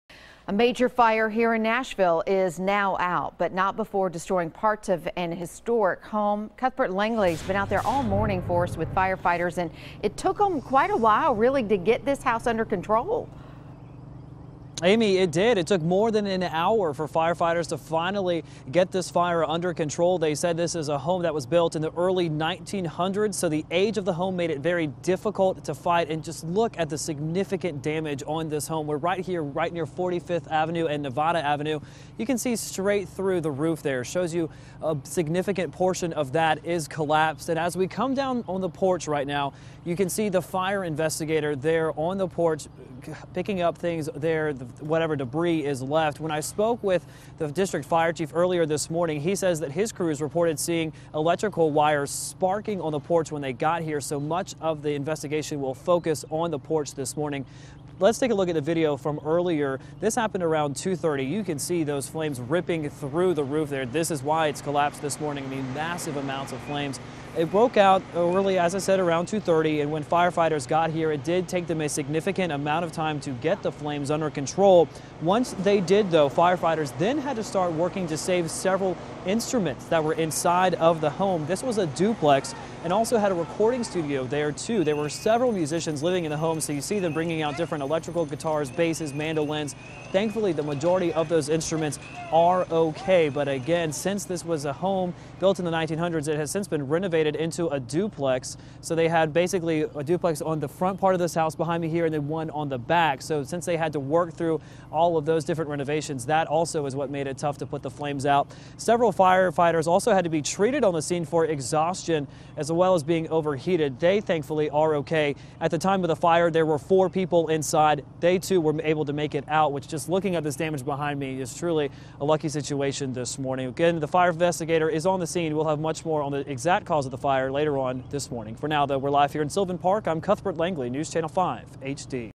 Early 1900's Home Destroyed In Fire On Nevada Avenue
A large early morning fire destroyed an historic home on Nevada Avenue.